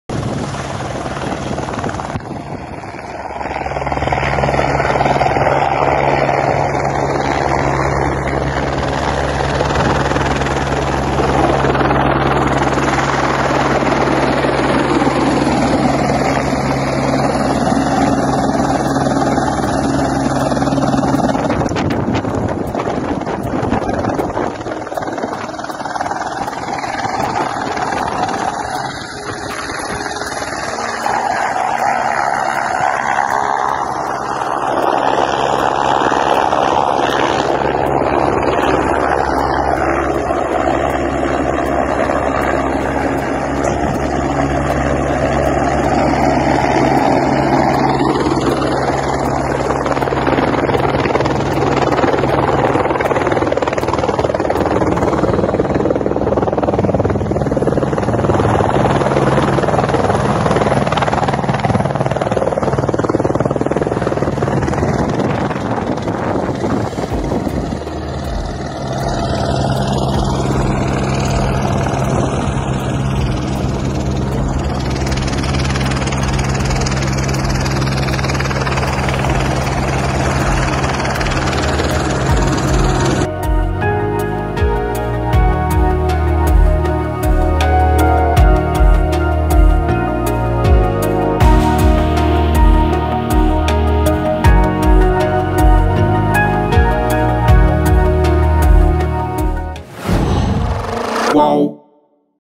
Helicóptero que caiu com 280kg de cocaína pertence a policial do DF
O helicóptero modelo Robinson 44 que se acidentou em Poconé (MT) está registrado em nome do policial Ronney José Barbosa Sampaio, do DF. No local da queda, foram encontrados 280 kg de cocaína.